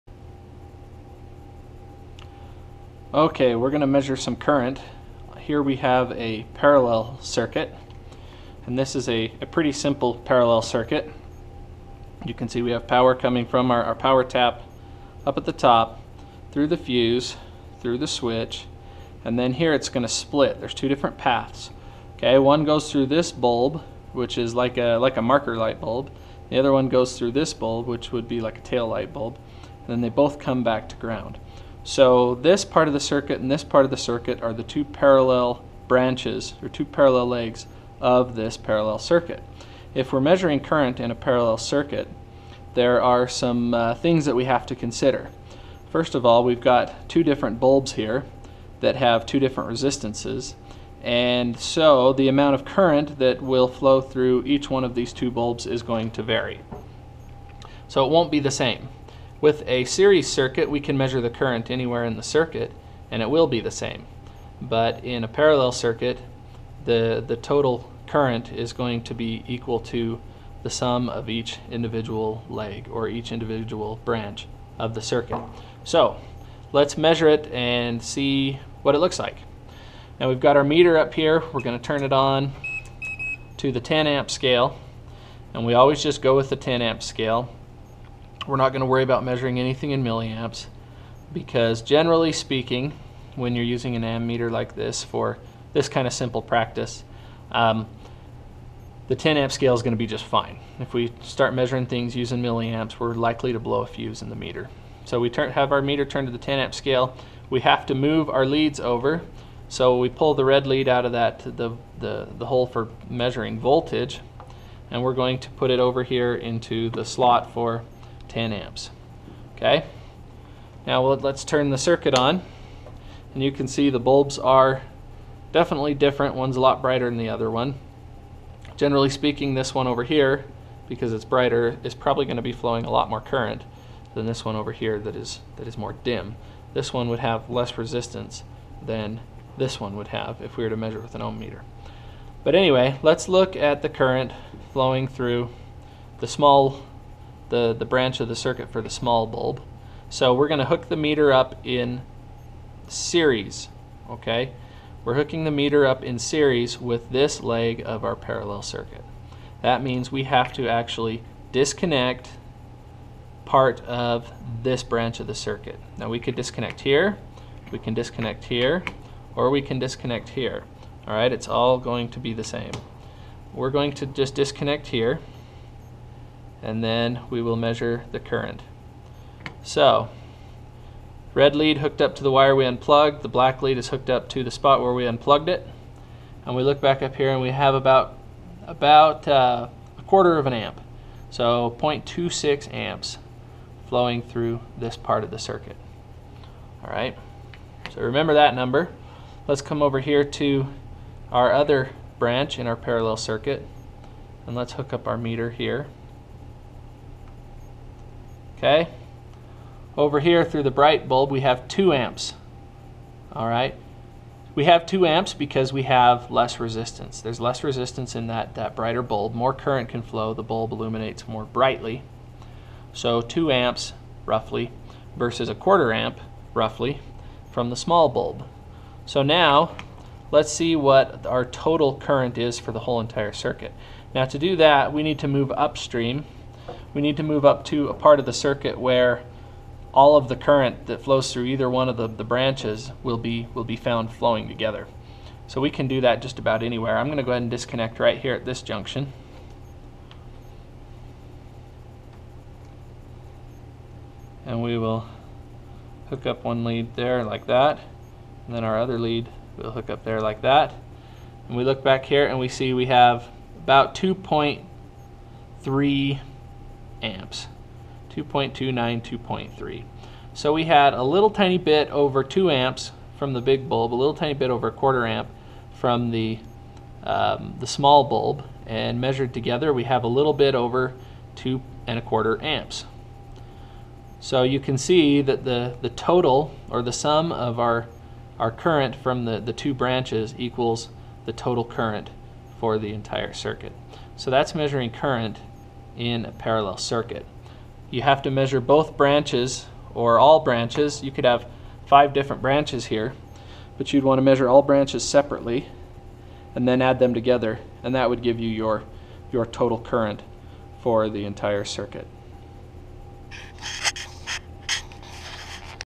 Using an Ammeter with a Parallel Circuit
Some thing to remember when using an ammeter to measure current in a parallel circuit.